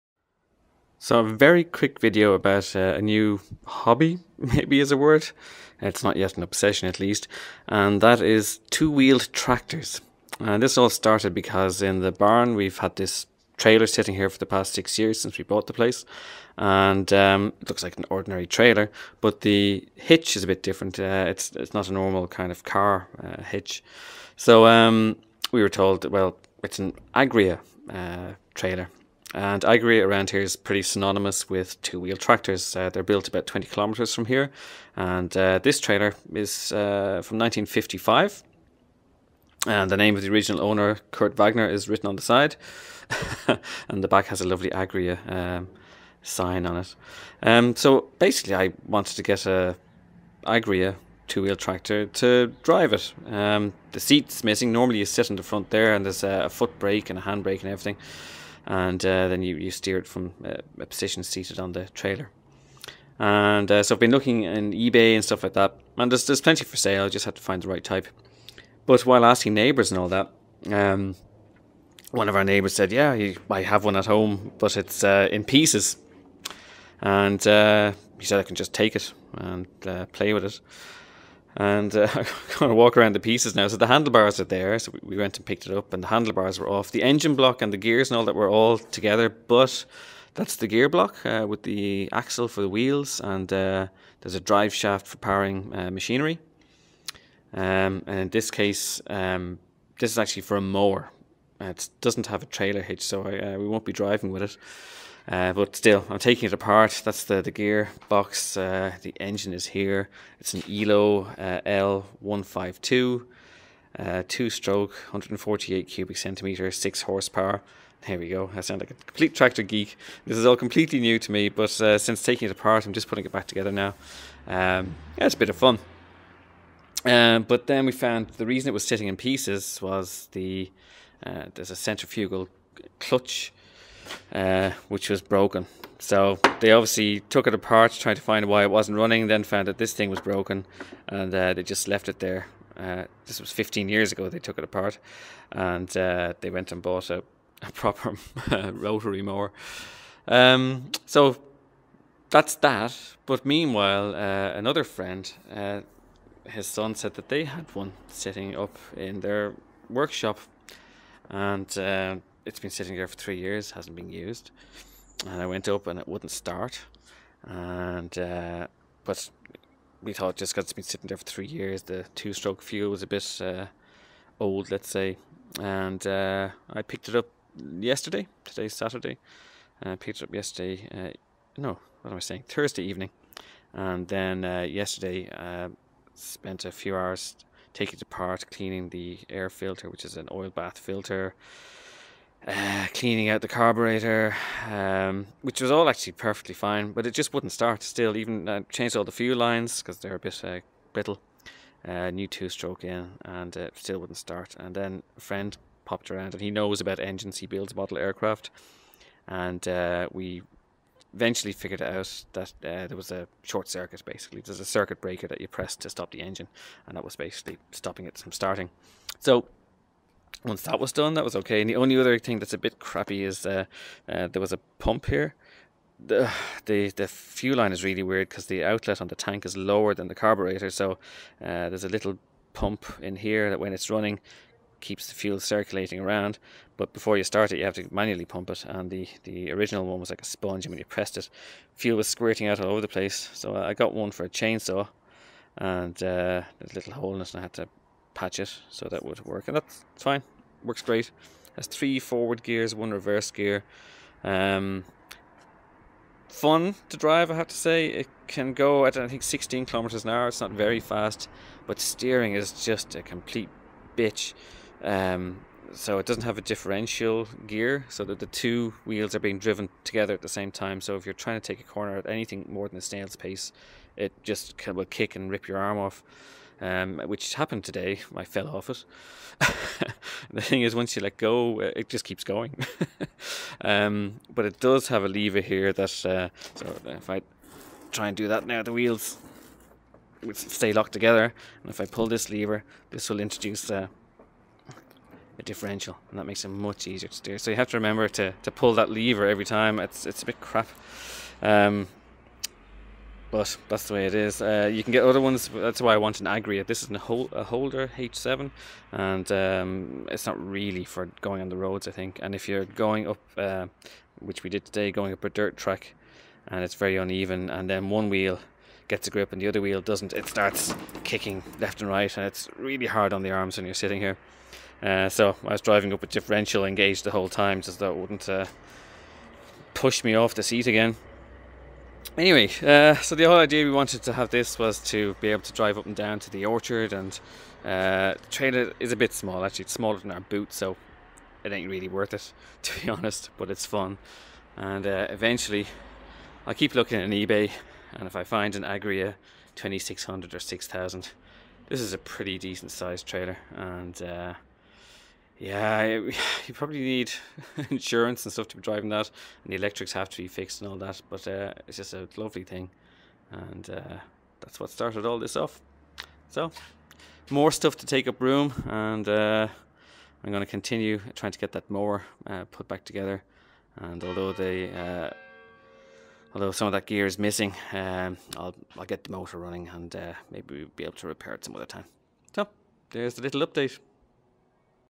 The Bauernhaus 2016-10-01: Two wheel tractors
A quick intro to a new hobby, of sorts. Two-wheel tractors. In this case, a Holder H7, which works, and a Holder M6 bar mower, which I am in the process of reassembling.